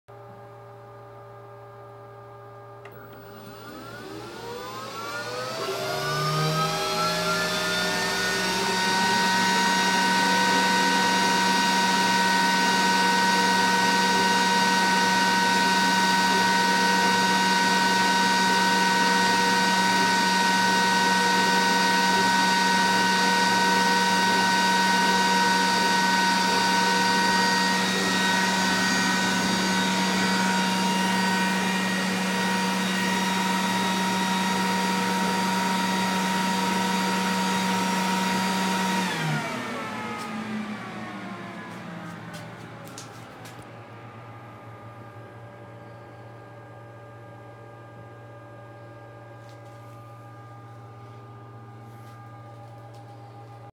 Glowforge Orange Light Error
My warranty replacement Glowforge is now interrupting prints with frequent orange error lights. This was my second attempt at this cut. Both failed within a few seconds.  The Glowforge had not been operating for a couple of days, so this is not an overheating issue.

UPDATE:
Glowforge is replacing my Pro after I uploaded logs showing that the tube cooling pump appears to have malfunctioned and that the print head is occasionally losing connectivity.